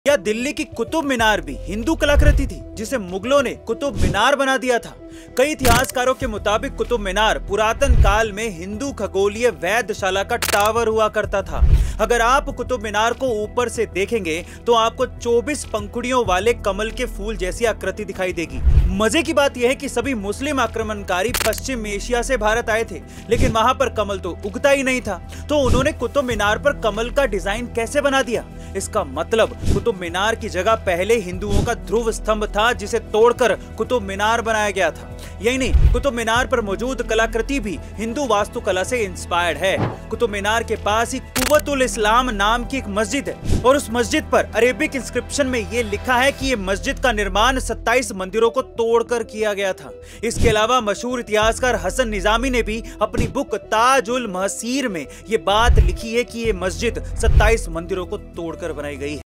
क़ुतुब मीनार कैसे थी हिंदू कलाकृति ? मुग़लों की साज़िश ! by arvindarora
Was Delhi's Qutub Minar also a Hindu artwork? Which was converted into Qutub Minar by the Mughals? According to many historians, Qutub Minar used to be the tower of Hindu Astronomical Observatory in ancient times. If you look at Qutub Minar from above, you will see a shape like a lotus flower with 24 petals! The interesting thing is that all the Muslim invaders came to India from West Asia, but lotus does not grow there! So how did they make the lotus design on Qutub Minar? This means that in place of Qutub Minar, earlier there was the Dhruv Pillar of Hindus, which was broken and Qutub Minar was built. Not only this, the artwork present on Qutub Minar is also inspired by Hindu architecture. There is a mosque named "Quwat Ul Islam" near Qutub Minar. And in the Arabic inscription on that mosque it is written that this mosque was built by demolishing 27 temples. Apart from this, famous historian Hasan Nizami has also written in his book "Taj ul Mahseer" that this mosque was built by demolishing 27 temples.